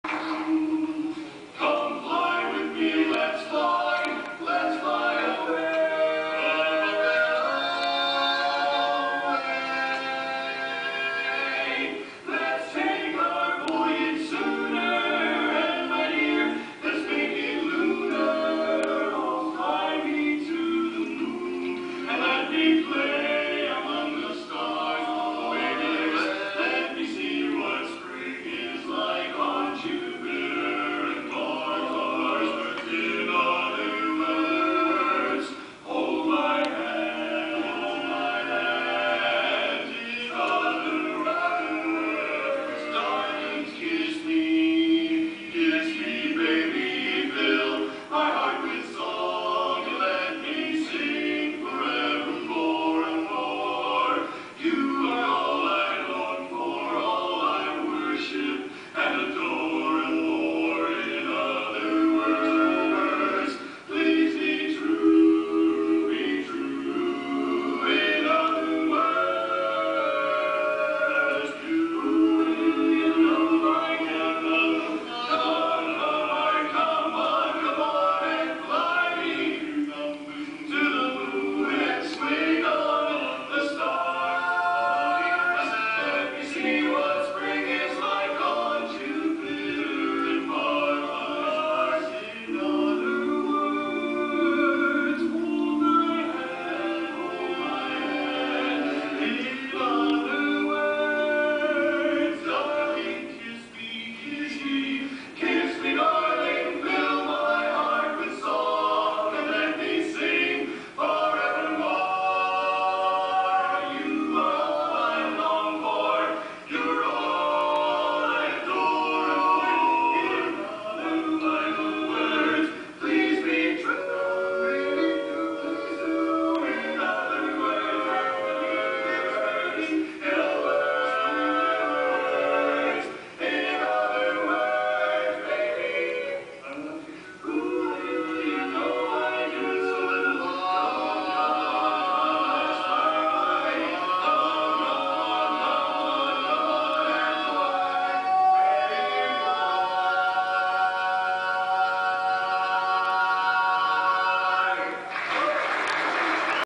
M-AD Classic quartets - BSQ
BSQ, out of retirement (shows only) doing a recent show.  BSQ were Intl medallists several times.